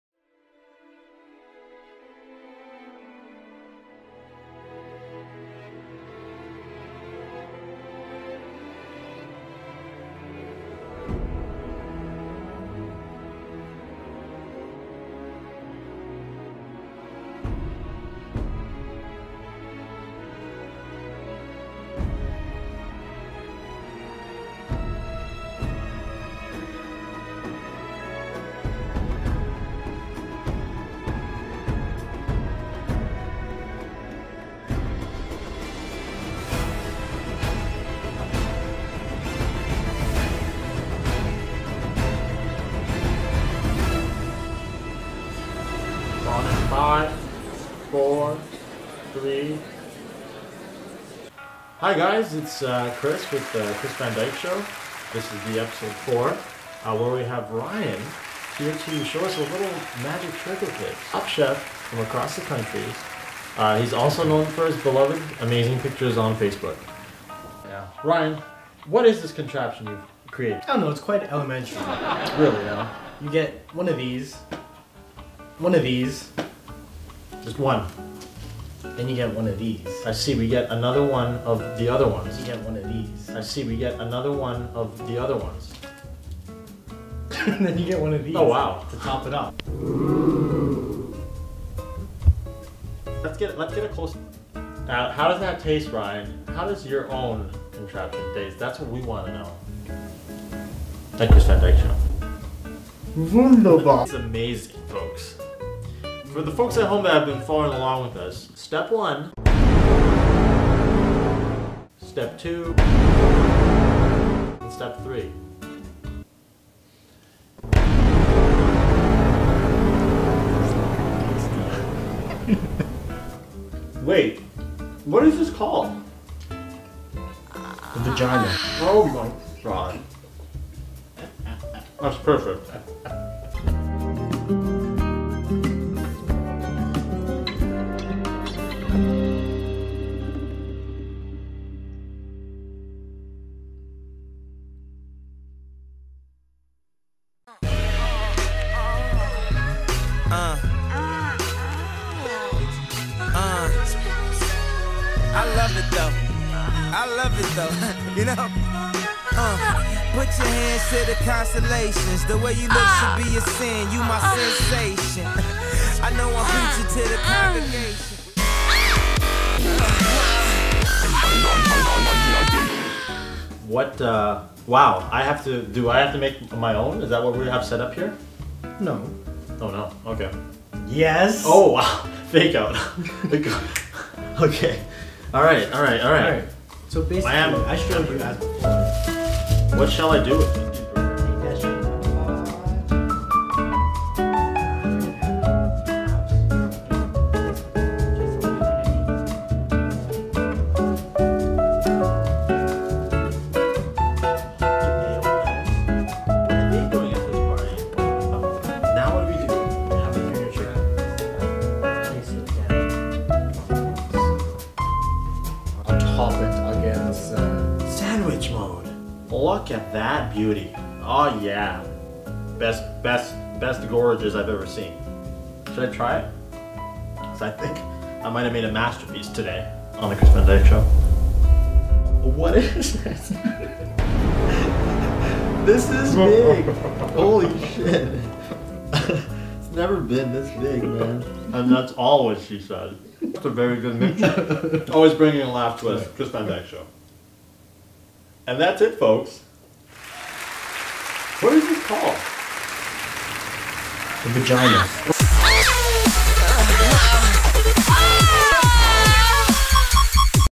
DuMTops : Chris Van Dyke
DuMTops are bonus shorts from the team behind the series, DMTSoup.

A culinary television series 'The Chris Van Dyke Show' hosted by Chris Van Dyke brings you the latest and tastiest delicacies from Tom Foolery; the greatest chef that ever lived.